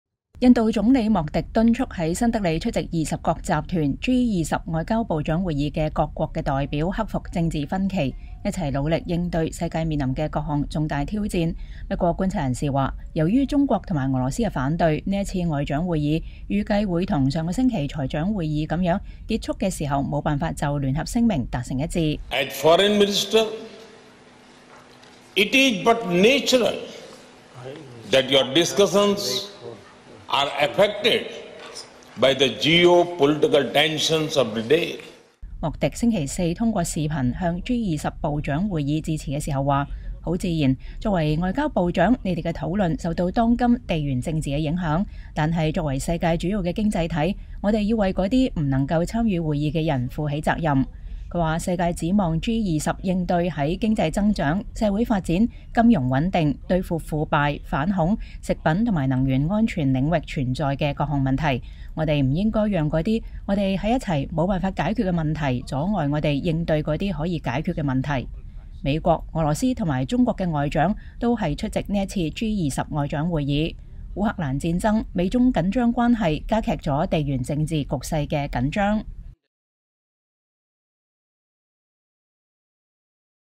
美中俄外長G20會議上現身 印度促各方克服政治分歧
印度總理莫迪敦促在新德里出席20國集團G20外交部長會議的各國代表克服政治分歧，一起努力應對世界面臨的各項重大挑戰。不過，觀察人士們說，由於中國和俄羅斯的反對，這次外長會議預計會跟上星期財長會議那樣，結束時無法就聯合聲明達成一致。